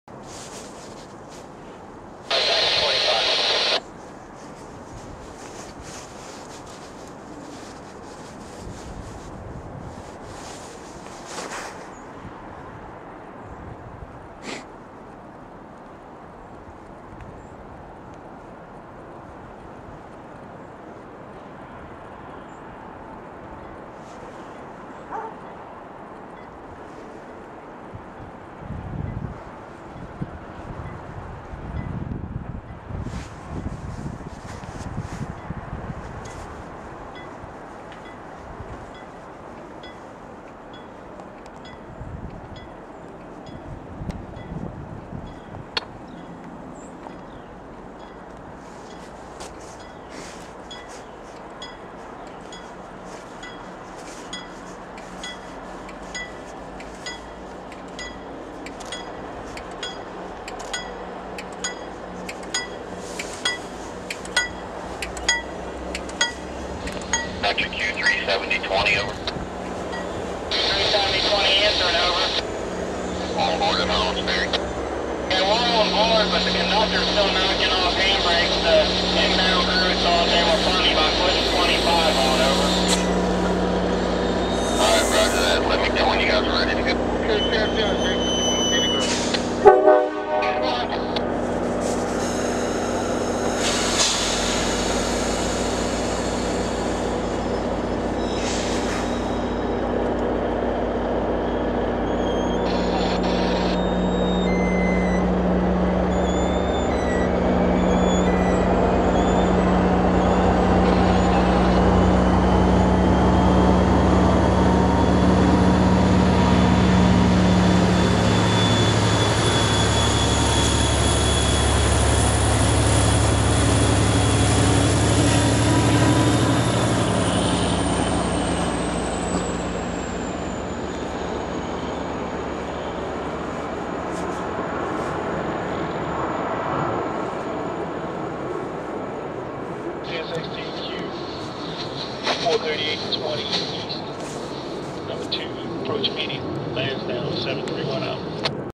MARC WB P-851 Snowshot
MARC (CSX) P-851 comes out of Baltimore WB, slowly passing through St. Denis without a needed station stop. Cab Car 7753 is up front with GP-39 73 pushing on the rear.